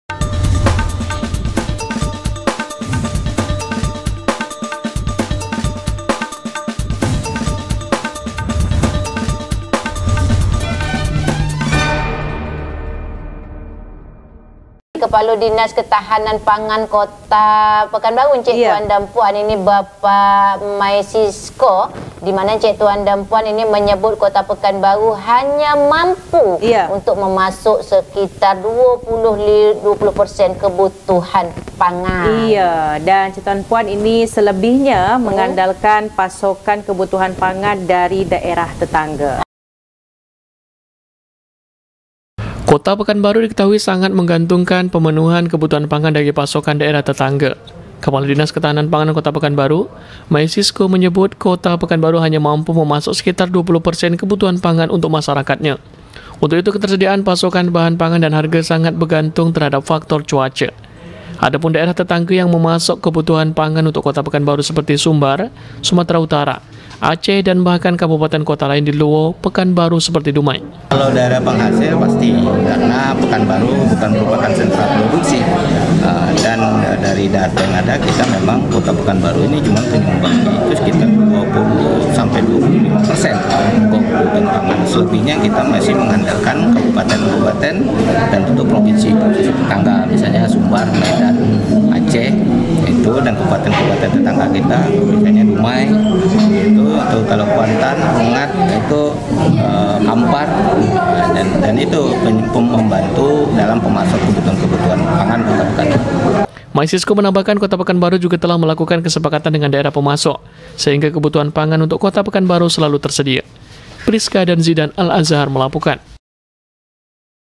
PEKANBARU HANYA MAMPU HASILKAN 20 PERSEN KEBUTUHAN PANGAN
Pekanbaru, riautelevisi.com- Kepala Dinas Ketahanan Pangan Kota Pekanbaru, Maisisco menyebut Kota Pekanbaru hanya mampu memasok sekitar 20% kebutuhan pangan, selebihnya mengandalkan pasokan kebutuhan pangan dari daerah tetangga.

Kota Pekanbaru diketahui sangat menggantungkan pemenuhan kebutuhan pangan dari pasokan  daerah tetangga. Kepala Dinas Ketahanan Pangan Kota Pekanbaru, Maisisco menyebut, Kota Pekanbaru hanya mampu memasok sekitar 20% kebutuhan pangan untuk masyarakatnya. Untuk itu ketersediaan pasokan bahan pangan dan harga sangat bergantung terhadap faktor cuaca. Adapun daerah tetangga yang memasok kebutuhan pangan untuk Kota Pekanbaru seperti Sumbar, Sumut, Aceh dan bahkan kabupaten kota lain di luar Pekanbaru seperti Dumai.

Maisisco menambahkan, Kota Pekanbaru juga telah melakukan kesepakatan dengan daerah pemasok, sehingga kebutuhan pangan untuk Kota Pekanbaru selalu tersedia.